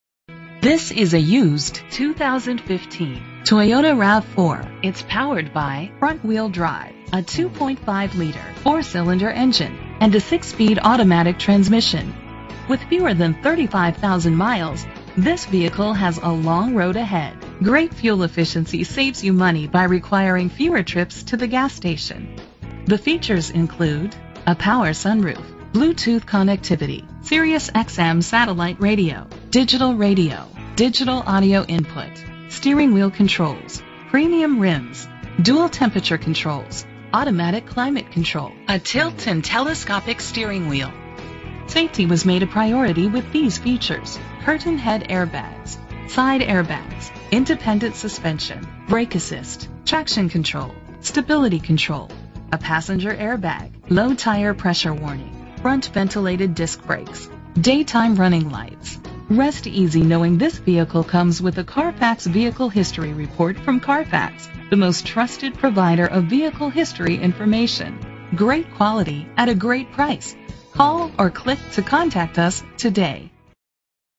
2015 Toyota RAV4 - Pensacola FL
Year: 2015
Make: Toyota
Model: RAV4
Trim: XLE
Engine: 2.5 liter inline 4 cylinder 16 valve
Transmission: 6-Speed Automatic
Color: Super White
Mileage: 31356
Address:
230 Beverly Parkway